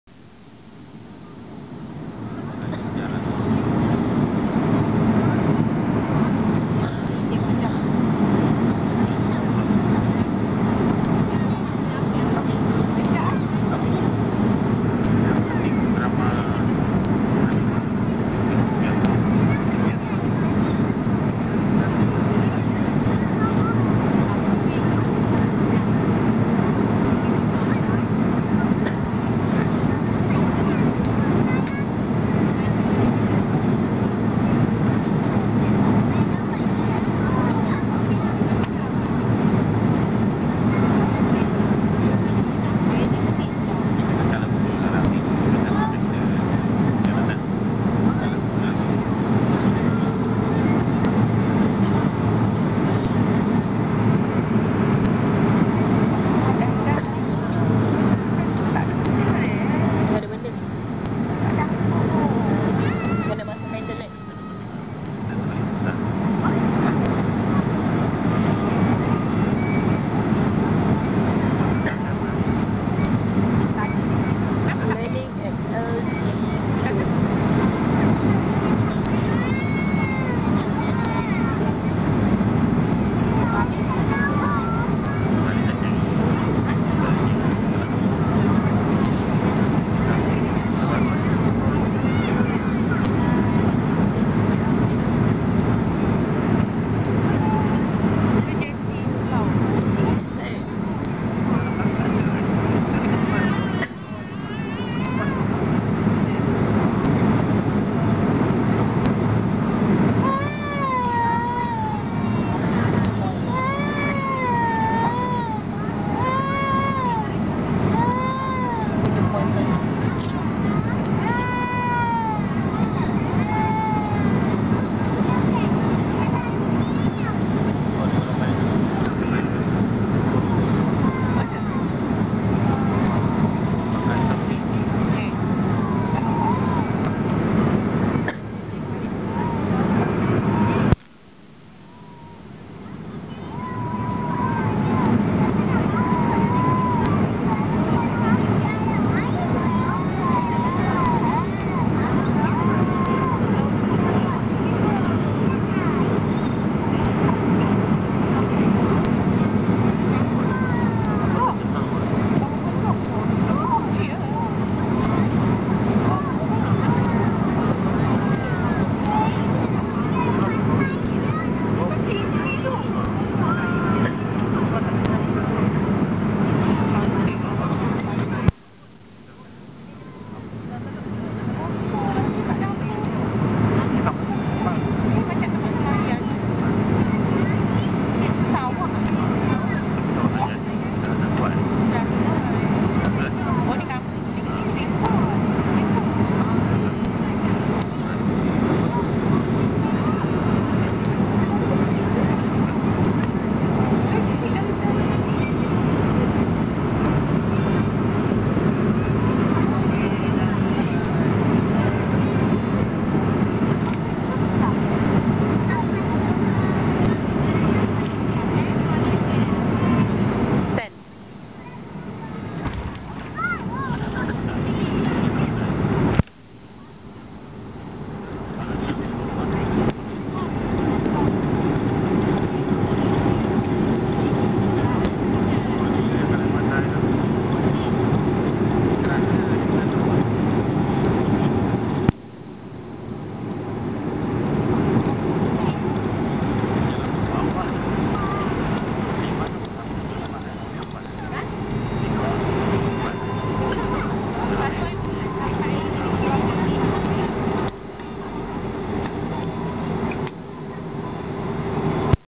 Malaysia Airlines B737-400 land at LGK(LANGKAWI)
malaysia airlines flight 1440 land at langkawi island(13dec09). KUL-LGK
it is nice landing..
this video has 2 parts and this is last parts.